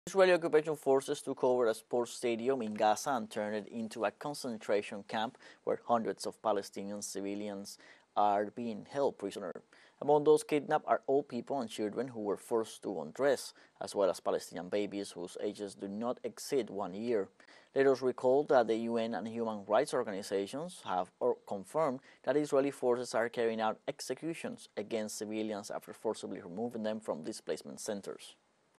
Israeli military strip and detain Palestinians in Gaza stadium
A video has emerged showing Israeli soldiers in Beit Lahia, northern Gaza, detaining more than 100 Palestinian men at gunpoint and forcing them to strip down to their underwear as they stand in a line and kneel on the ground. teleSUR